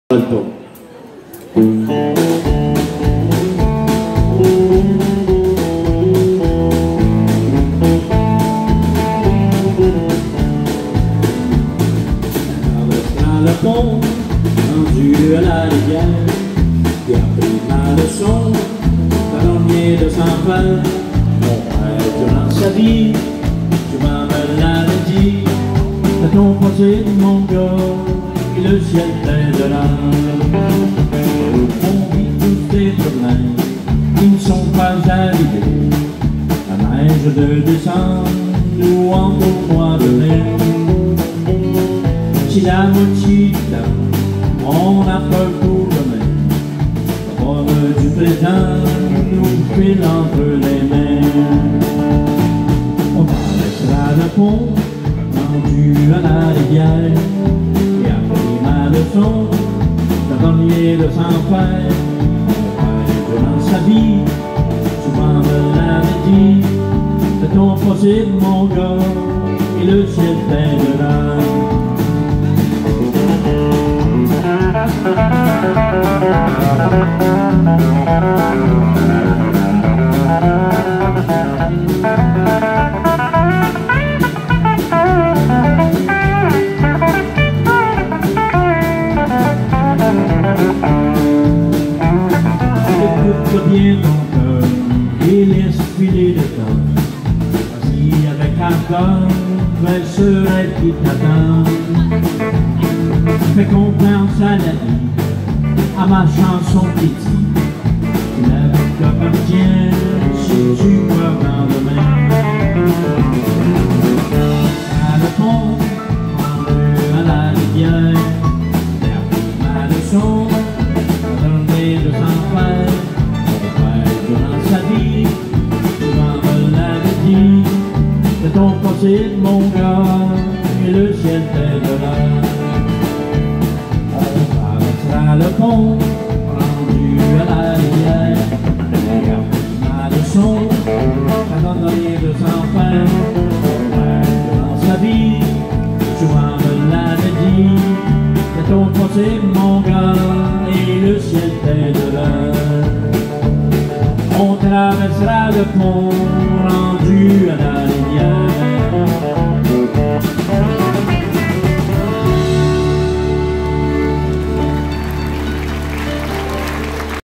Bernard Paquette, lors du Festival de Steeve Desmarais 2018 à Notre-Dame de Pierreville
Vidéos fait par le Ti-Bout, lors du Festival de Steeve Desmarais 2018 à Notre-Dame de Pierreville ... Ne pas oublier de vous abonnez à ma chaine YouTube gratuite, en cliquant sur ma petite photo en bas à droite, merci d'avance ...